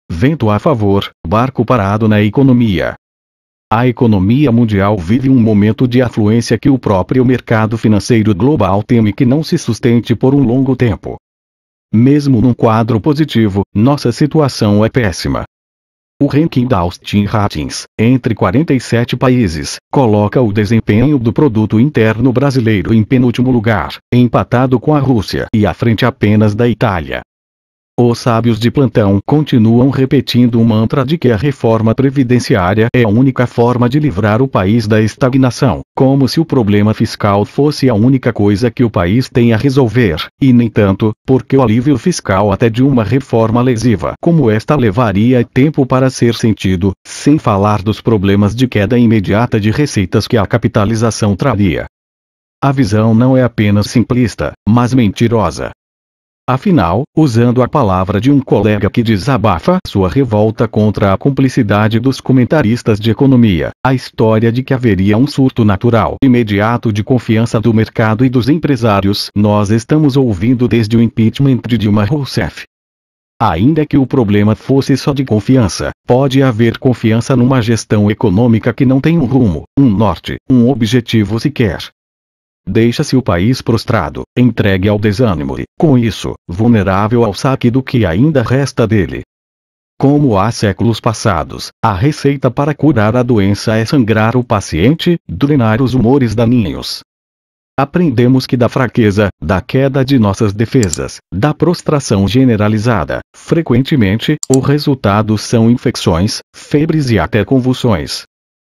Vento a favor, barco parado na economia
A economia mundial vive um momento de afluência que o próprio mercado financeiro global teme que não se sustente por um longo tempo.
Mesmo num quadro positivo, nossa situação é péssima.
Um ranking da Austin Ratings, entre 47 países, coloca o desempenho do Produto Interno Brasileiro em penúltimo lugar, empatado com a Rússia e à frente apenas da Itália.